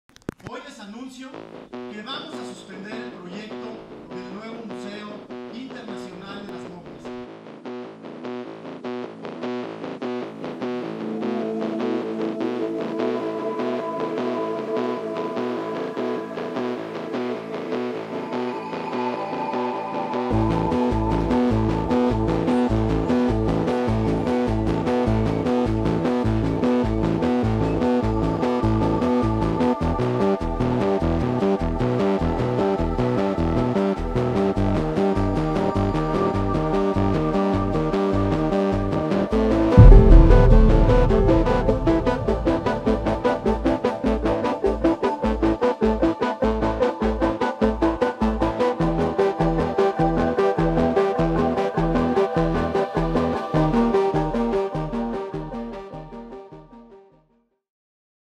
No fui yo, fue la 4T: Navarro se rinde con las momias
Alcalde de Guanajuato intenta convertir en reto político la gestión fracasada de un nuevo museo que nunca convenció a la ciudadanía de la capital y la movilizó en oposición.